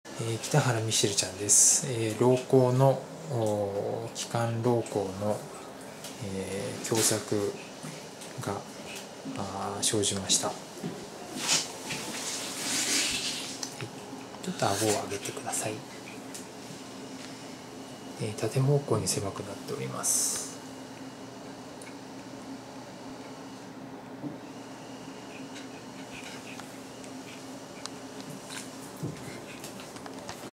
Laryngeal collapse#2-permanent tracheostomy-DAY83
Laryngeal collapse stage3 was diagnosed in the dog. Permanent tracheostomy was performed.On DAY83, slightly stenosis of tracheastorma occurred.